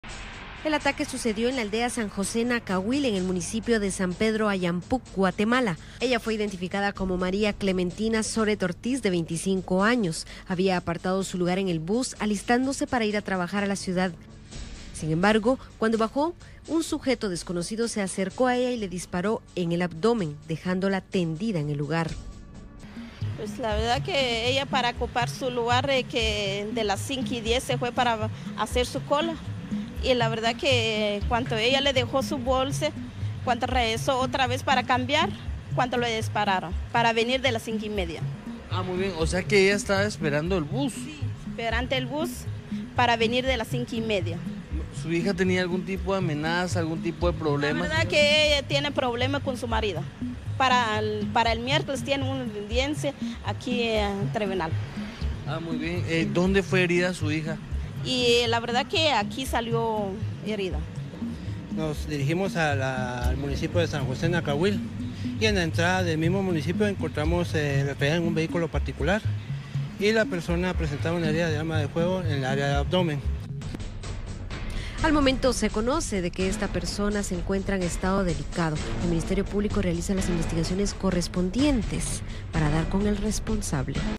Mujer es atacada en San Pedro Ayampuc
Víctima se debate entre la vida y la muerte